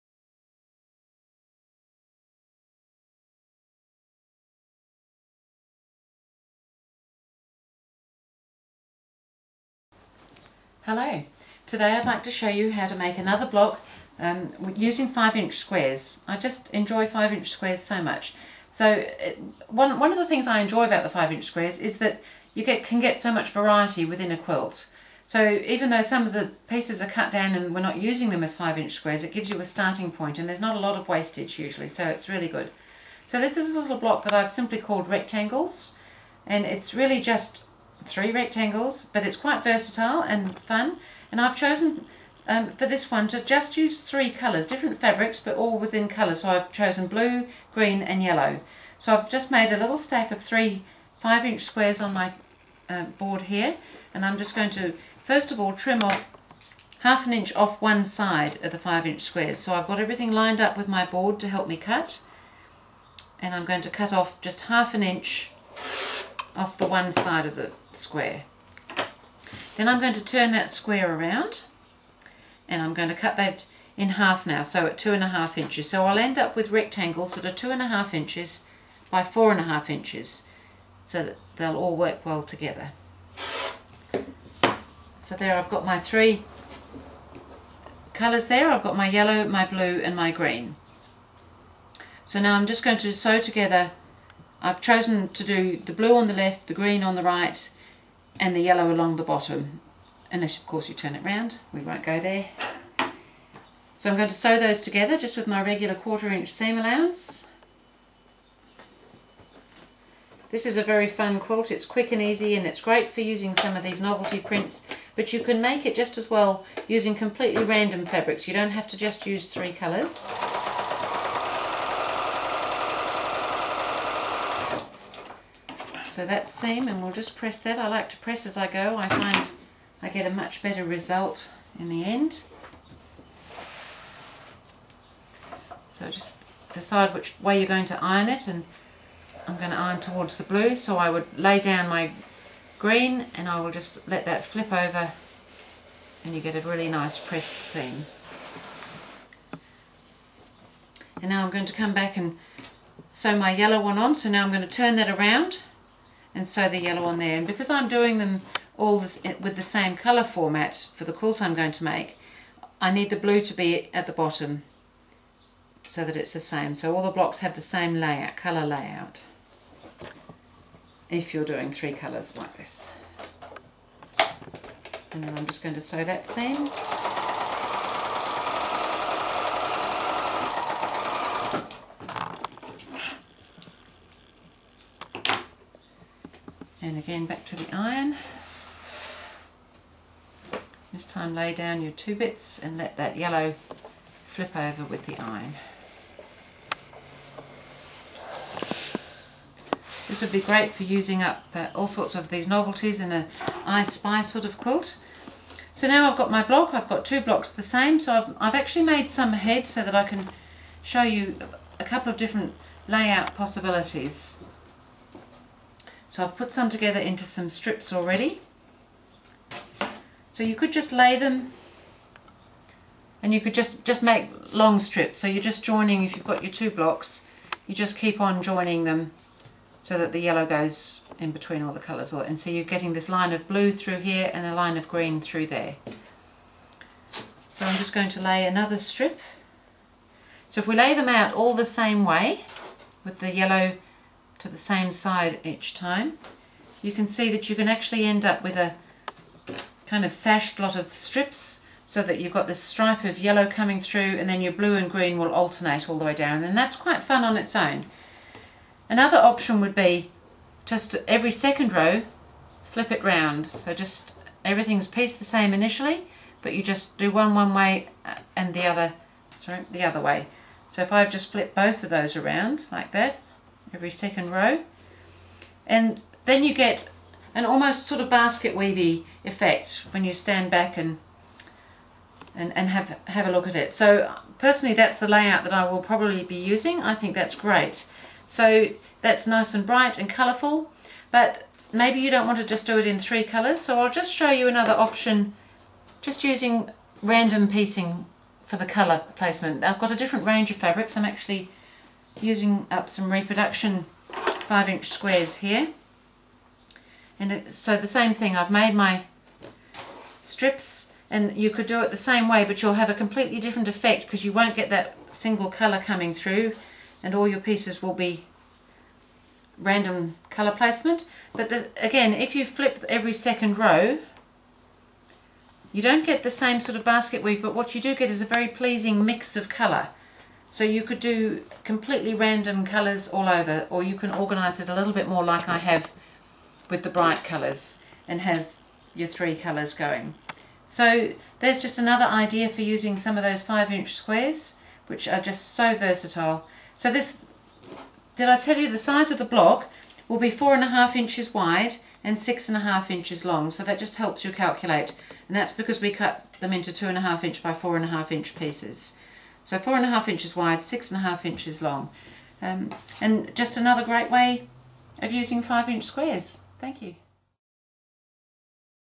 How to make a Rectangles Block using 5" squares - Quilting Tips & echniques 047
In this video I show you how to make a simple Rectangles block using 5" squares. This is a great block for using up a variety of different colours or making a simple 3-colour quilt.